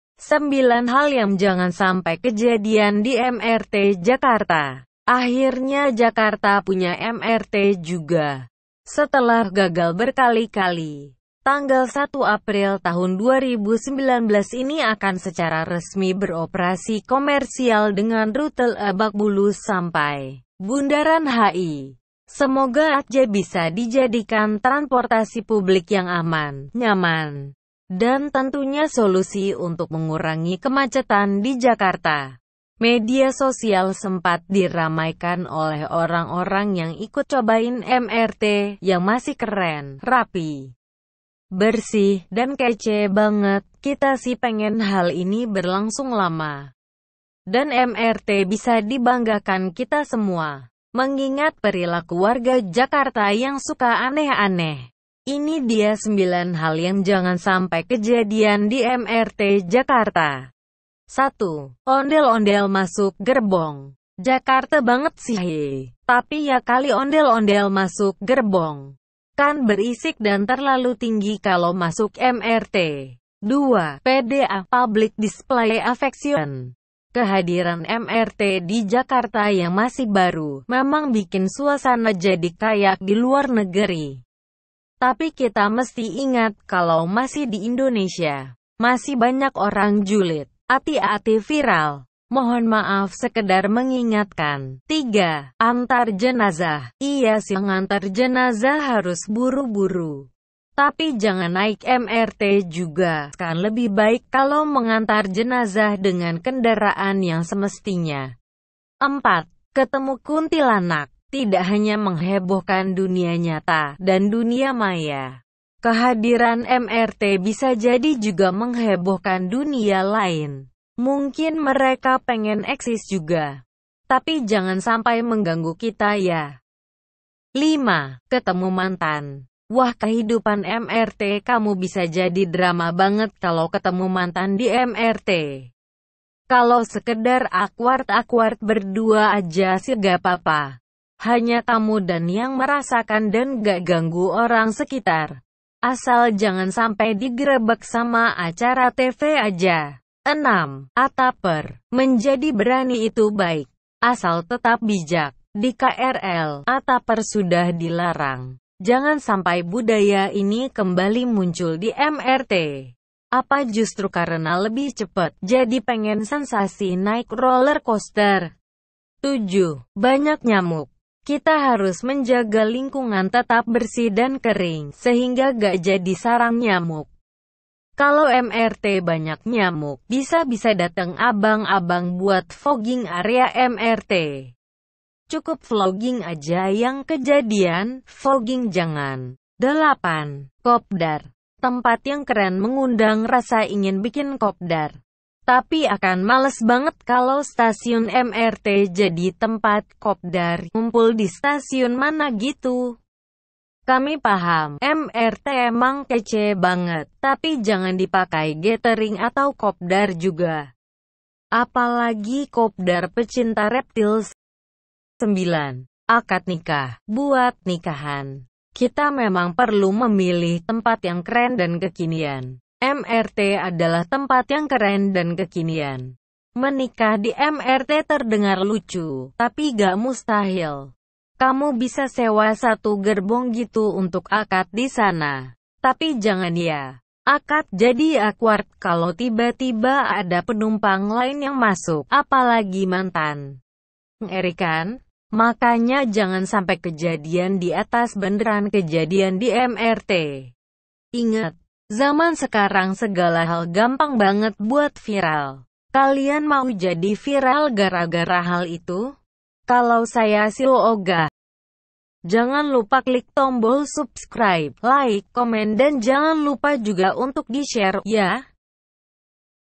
9 Hal yang Jangan Sampai Kejadian di MRT Jakarta
Akhirnya Jakarta punya MRT juga, setelah gagal berkali-kali. Tanggal 01 April 2019 ini akan secara resmi beroperasi di Jalur Bunderan HI sampai Lebak Bulus. Semoga aja bisa dijadikan tranportasi publik yang aman, nyaman, dan tentunya solusi untuk mengurangi kemacetan di Jakarta.

Media sosial sempat diramaikan oleh orang-orang yang ikut cobain MRT, yang masih keren, rapi, bersih, dan kece banget. kecer sih pengen hal ini berlangsung lama, dan MRT bisa dibanggakan kita semua. Mengingat perilaku warga Jakarta yang suka aneh-aneh, ini dia 9 hal yang jangan sampai kejadian di MRT Jakarta.

1. Ondel-ondel Masuk Gerbong
Jakarte banget sih ye. Tapi ya kali ondel-ondel masuk gerbong, kan berisik dan terlalu tinggi kalo masuk MRT.

2. PDA (Public Display Affection)
Kehadiran MRT di Jakarta yang masih baru, memang bikin suasana jadi kayak di luar negeri, tapi kita mesti ingat kalau masih di Indonesia. Masih banyak orang julid. Ati-ati viral. Mohon maaf sekedar mengingatkan.

3. Antar Jenazah pakai MRT
Iya sih nganter jenazah harus buru-buru, tapi jangan naik MRT juga, kan lebih baik kalau mengantar jenazah dengan kendaraan yang semestinya.

4. Ketemu Kuntilanak di MRT jakarta
Tidak hanya menghebohkan dunia nyata, dan dunia maya, kehadiran MRT bisa jadi juga menghebohkan dunia lain. Mungkin mereka pengen eksis juga, tapi jangan sampai mengganggu kita ya.

5. Ketemu Mantan di MRT Jakarta
Wah kehidupan MRT kamu bisa jadi drama banget kalau ketemu mantan di MRT. Kalo sekedar awkward-awkward berdua aja sih gapapa, hanya kamu dan yang merasakan dan gak ganggu orang sekitar. Asal jangan sampe di grebek sama acara TV aja.

6. Atapers di MRT jakarta
Menjadi berani itu baik, asal tetap bijak. Di KRL, Atapers sudah dilarang. Jangan sampai budaya ini kembali muncul di MRT. Apa justru karena lebih cepet, jadi pengen sensasi naik Roller Coaster?

7. Banyak Nyamuk di MRT jakarta
Kita harus menjaga lingkungan tetap bersih dan kering, sehingga gak jadi sarang nyamuk. Kalo MRT banyak nyamuk, bisa-bisa dateng abang-abang buat fogging area MRT. Cukup vlogging aja yang kejadian, fogging jangan.

8. Kopdar
Tempat yang keren mengundang rasa ingin bikin kopdar, tapi akan males banget kalo stasiun MRT jadi tempat kopdar, ngumpul di stasiun mana gitu. kita paham, MRT emang kece banget, tapi jangan dipakai gathering atau kopdar juga, apalagi kopdar pecinta reptil.

9. Akad Nikah di MRT jakarta
Buat nikahan, kita memang perlu memilih tempat yang keren dan kekinian. MRT adalah tempat yang keren dan kekinian. Menikah di MRT terdengar lucu, tapi gak mustahil. Kamu bisa sewa satu gerbong gitu untuk akad di sana. Tapi jangan ya! Akad jadi awkward kalo tiba-tiba ada penumpang lain yang masuk, apalagi mantan!

Ngeri kan? Makanya jangan sampai kejadian di atas beneran kejadian di MRT. Inget, jaman sekarang segala hal gampang banget buat viral! Kalian mau jadi viral gara-gara hal itu? kita sih ogah.